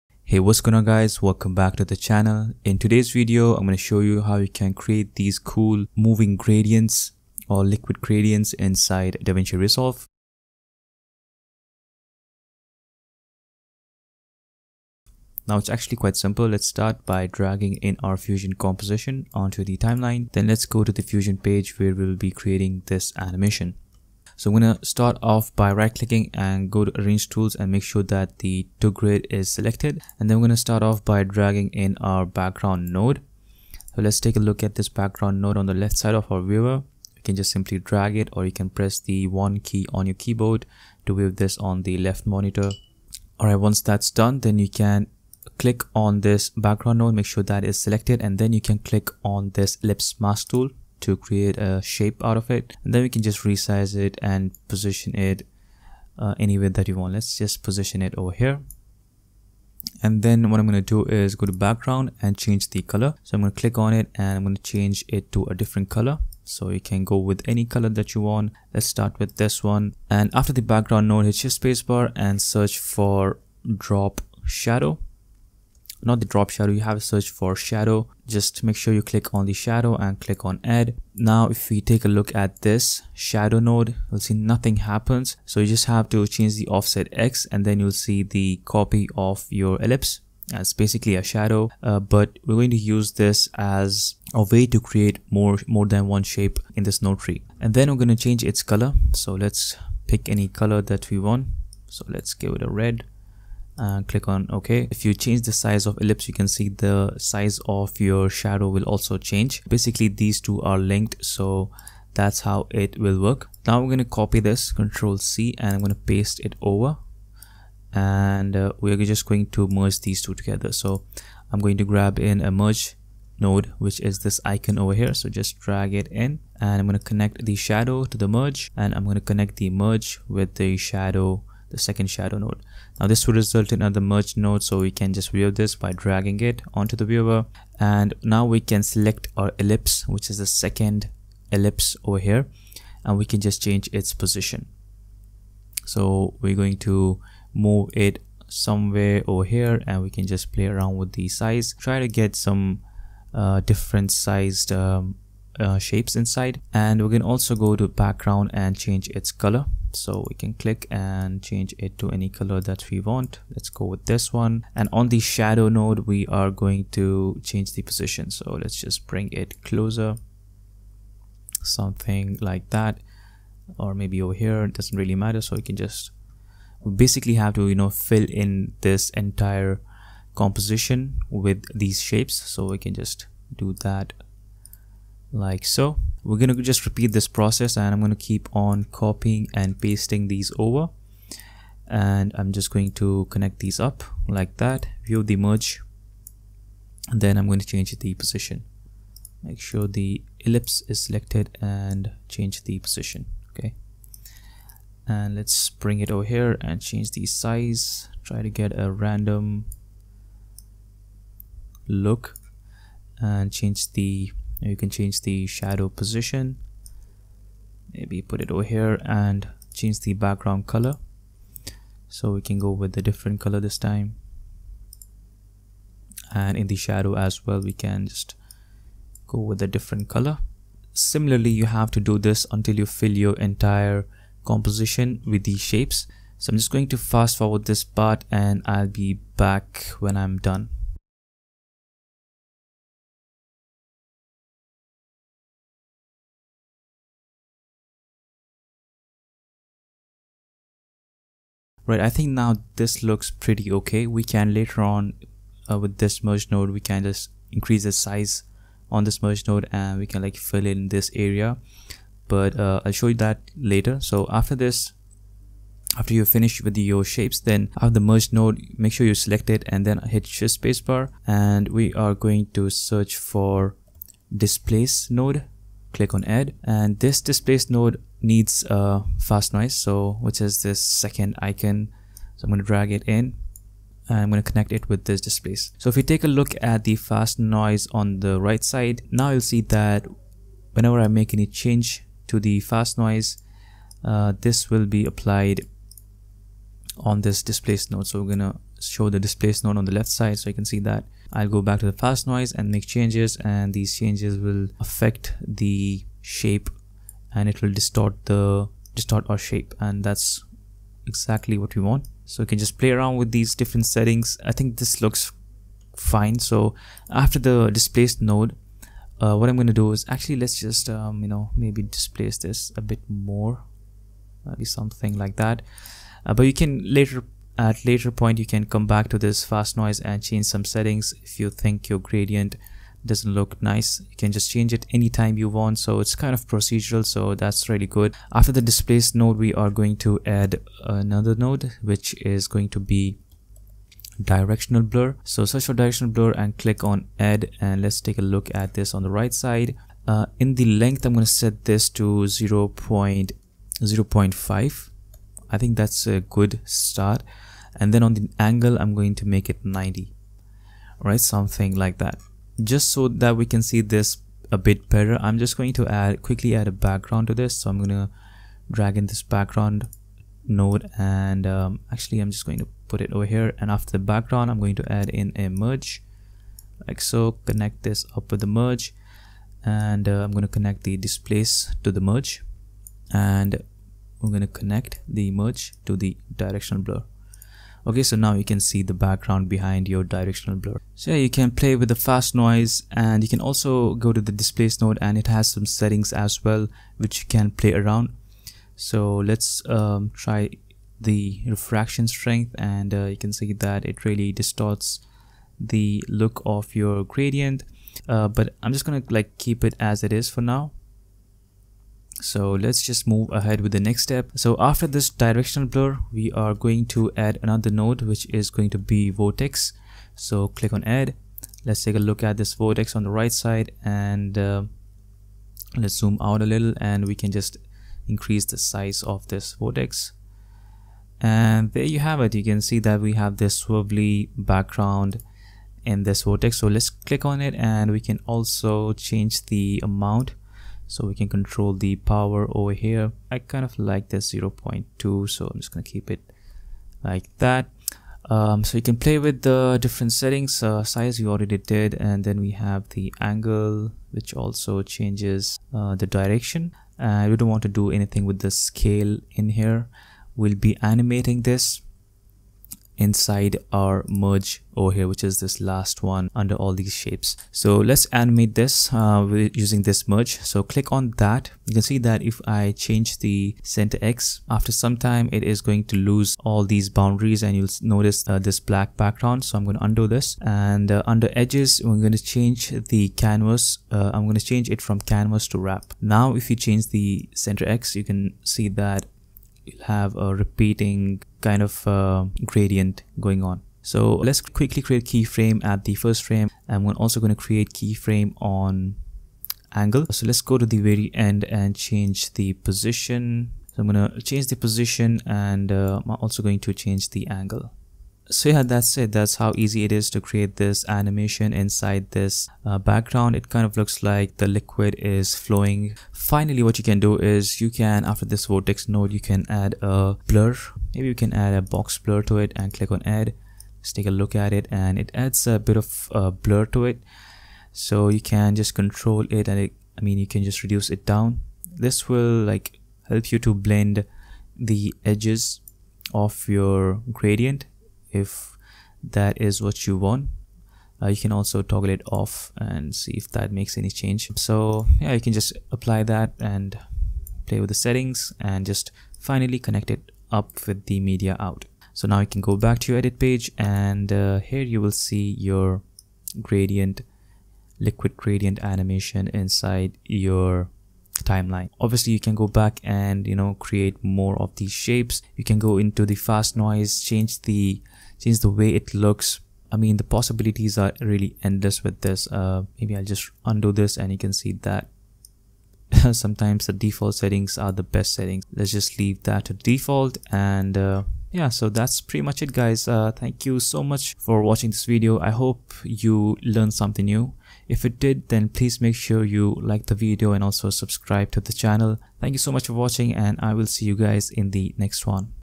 AMAZING Abstract Liquid Background - Davinci Resolve 17 Tutorial
In this video, we're going to create amazing looking abstract liquid backgrounds in Davinci Resolve 17. You can use these backgrounds in a variety of scenarios such as streaming, end screen, intro, VJ loops, etc. This effect is easy to create. If you don't want to create the entire animations from scratch then you can access the project file below.